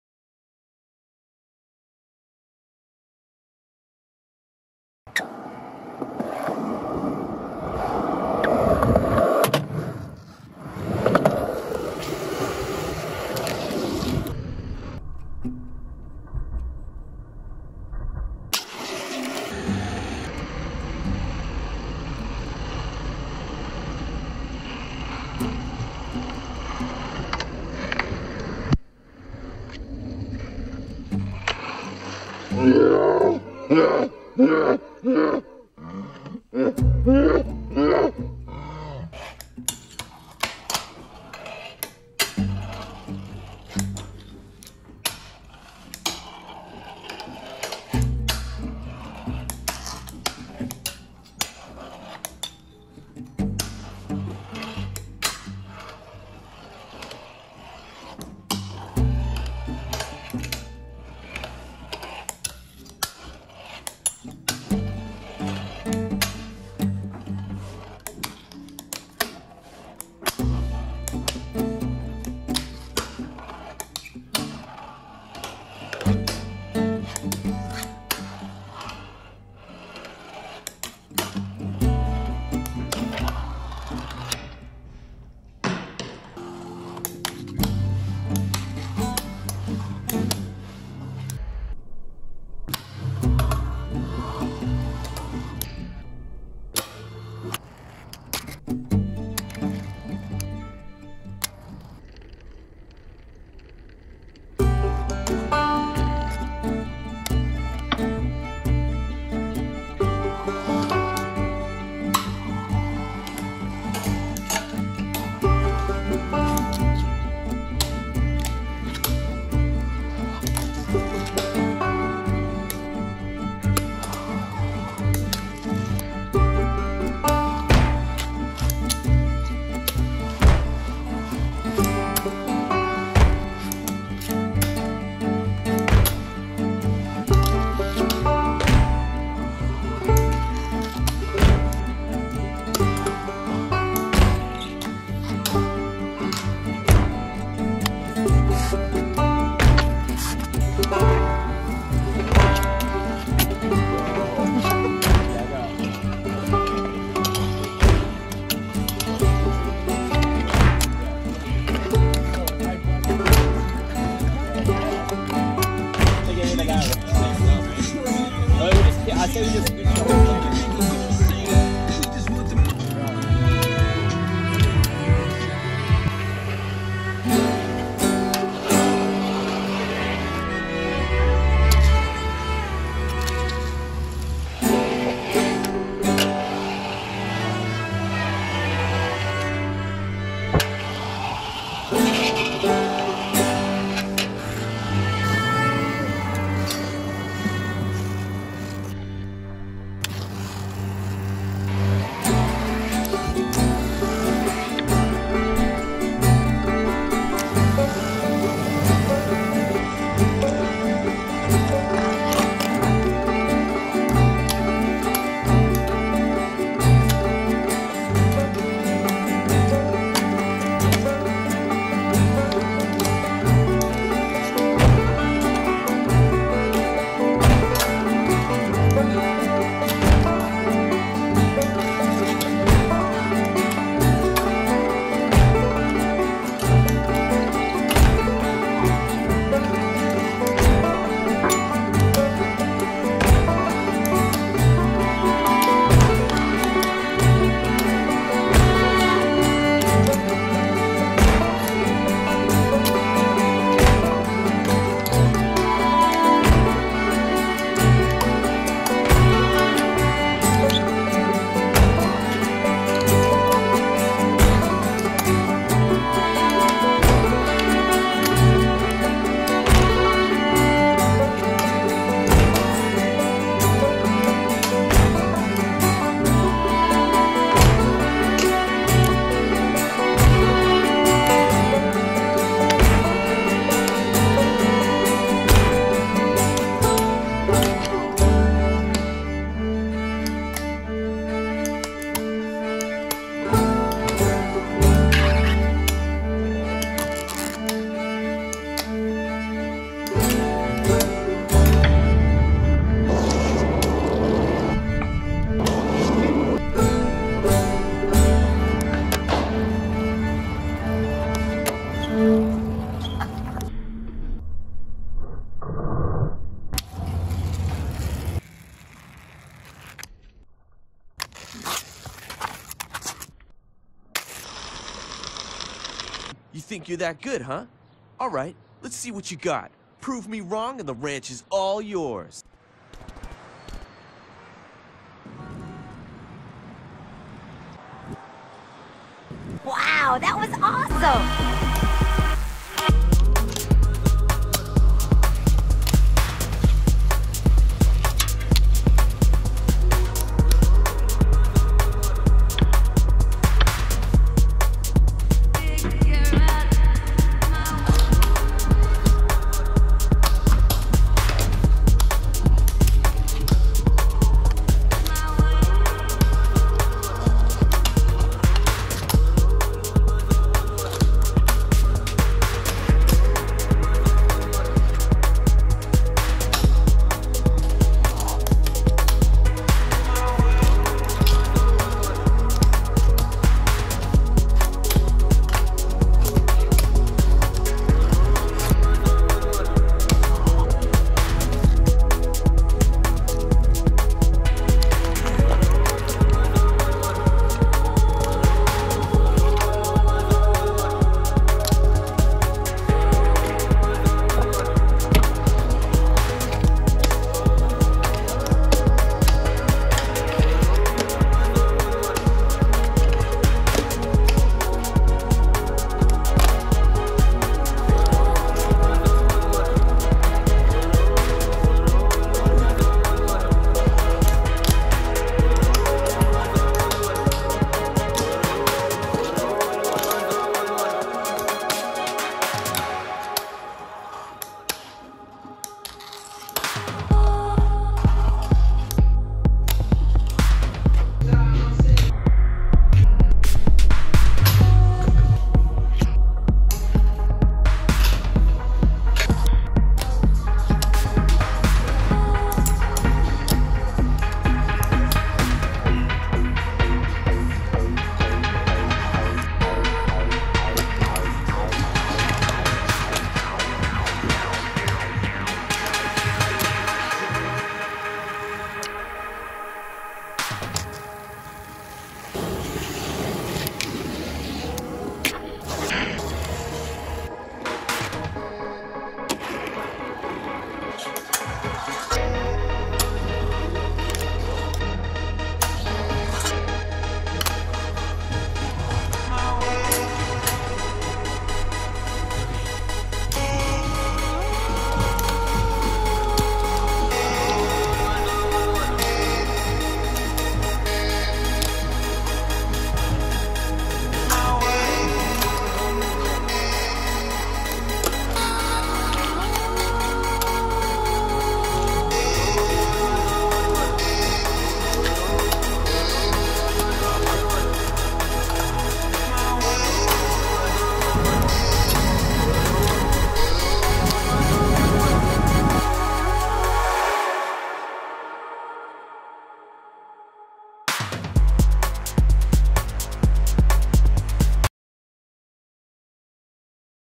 U4| A Full Length Fingerboard Film
I started "unfocused on reality" about 3 years ago. It seemed at the time when I made the first video that it was going to be the last. Immediately after sharing it with the world I knew that the next year I had to make another one. As you can imagine, the same thing happen over and over again. Now we are here, at 4. Maybe I'll do 5, I'm totally not sure, but what I do know is that I have alot of fun making these! Filming all over the place, finding cool new spots to try. Its all in good fun.

This one isn't as technical as the others, but its detailed in ways you can't put your finger on. It's deep, deeper than fingerboarding. I hope you guys enjoyed u4. Until next time                                                              -Eusi